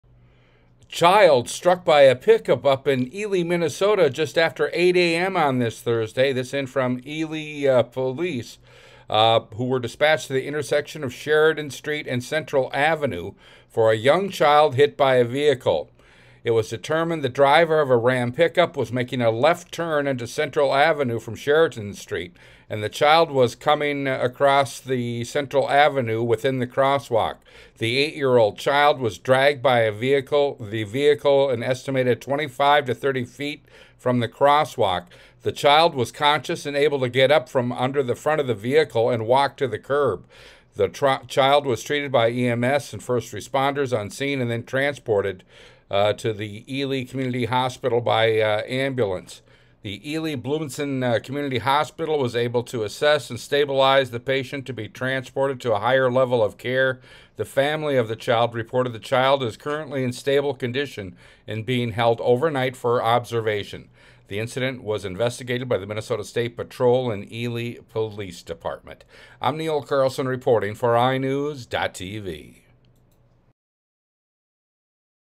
Child Hit By Pickup In Ely, Minnesota
ELY, MN (iNewZ.TV) A child was struck by a pickup in Ely, Minnesota Thursday morning.